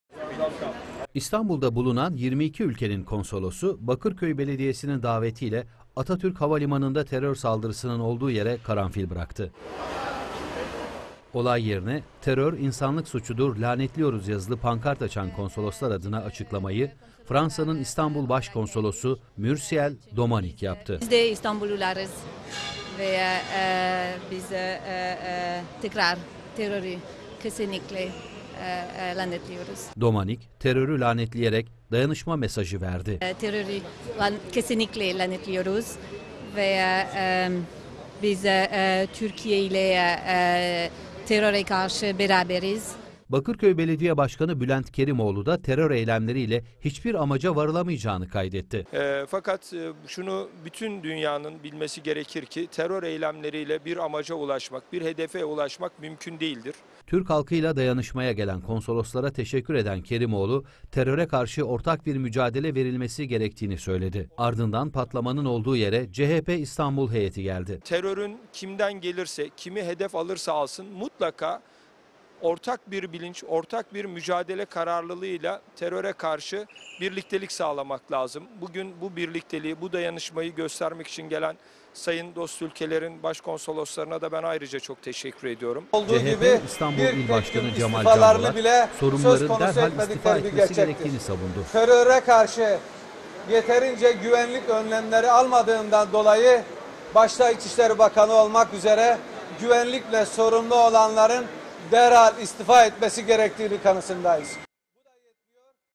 22 ülke konsolosu 44 kişinin hayatını kaybettiği saldırıyı kınadı
İstanbul'daki konsoloslar Atatürk Havalimanı'nda yaşanan terör saldırısında hayatını kaybedenlerin anısına havalimanına karanfil bıraktı. Terörün lanetlendiği anmada, Türkiye ile dayanışma mesajları verildi.